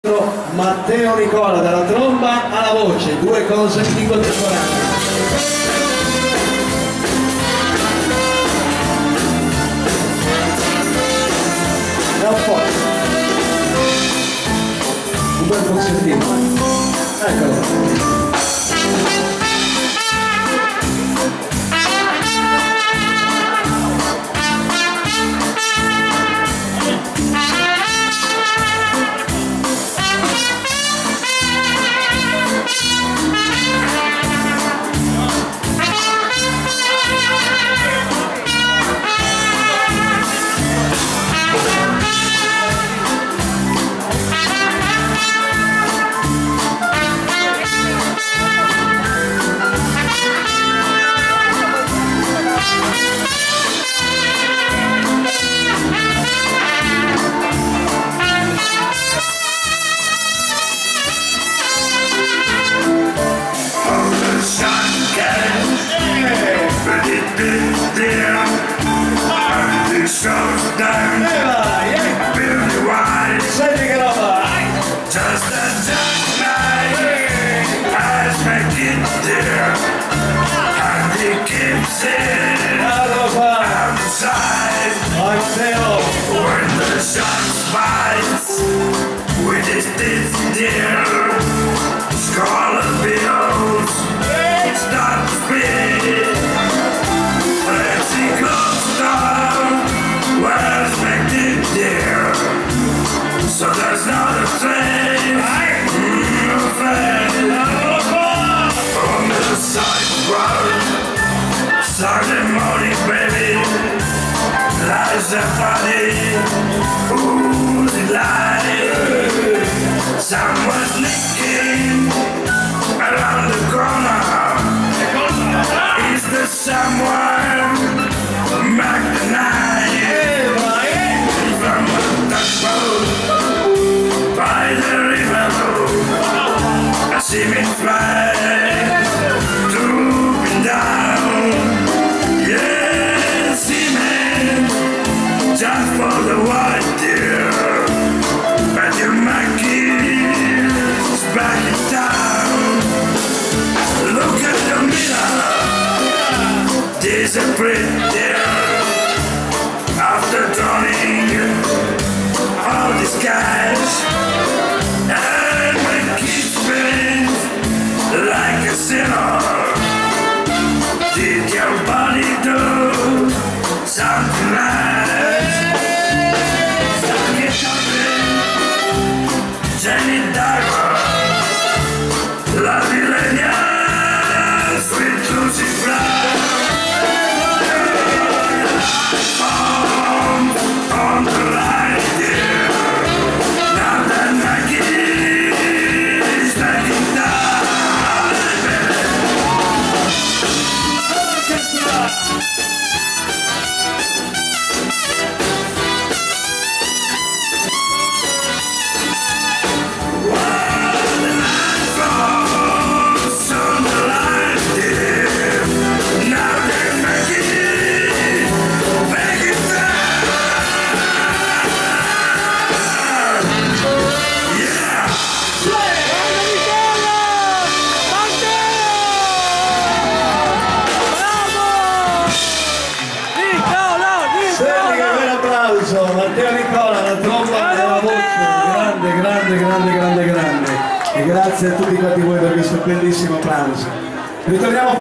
24-01-2018 DISCOTECA MELODI LA VOCE DI MATTEO NICOLA (FILMADOR RENATO)
FENOMENALE ORCHESTRA ITALIANA BAGUTTI LA NUMERO UNO D'ITALIA QUI STO RIPRENDENDO MENTRE CANTA MATTEO NICOLA UN NUMERO UNO SUONARE LA TROMBA MA ANCHE CANTARE E BRAVO COMPLIMENTI MATTEO UN ABBRACCIO FORTE CALOROSO DAL TUO AMICO SIMPATIZZANTE CON STIMA AMMIRAZIONE IL FILMADOR RENATO ZILIO E MIA MOGLIE SERENELLA DA PADOVA